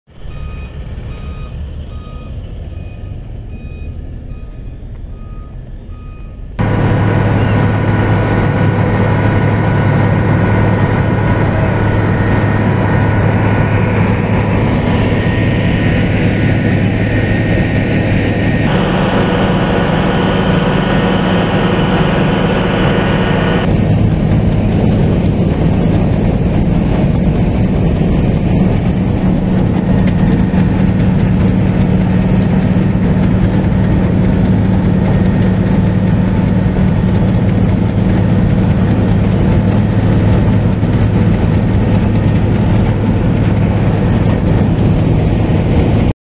Por reencarpetamiento cierran carril hacia Tlaxcala enFederal a Texmelucan a la altura de Villa Alta
▶️Por reencarpetamiento cierran carril hacia Tlaxcala en la Federal a Texmelucan a la altura de Villa Alta
Vía: Héctor Lorenzo// EsImagen Tlaxcala

Tepetitla,Tlaxcala.15-agosto-2023-Durante este día, comenzaron los trabajos de reencarpetamiento de la carretera federal Texmelucan-Tlaxcala, por lo cual, el carril con dirección hacia territorio tlaxcalteca se encuentra totalmente cerrado a la circulación. Cabe señalar que estas labores durarán algunos días, por lo cual los conductores deben de extremar precauciones.